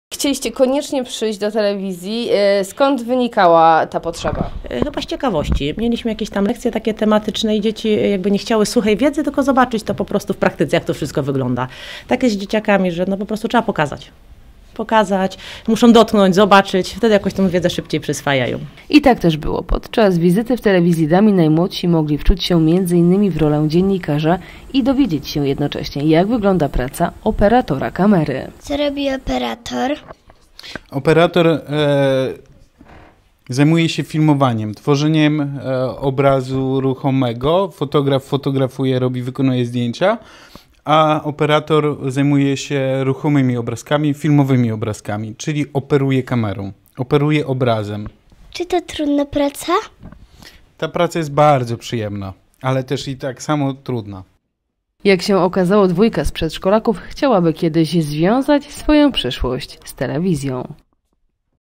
Przedszkolaki z wizytą w TV DAMI
Dziś obchodzony jest Światowy Dzień Telewizji – w związku z tym przedszkolaki z „Krainy Smyka” chciały dowiedzieć się więcej na temat tego jak ona działa.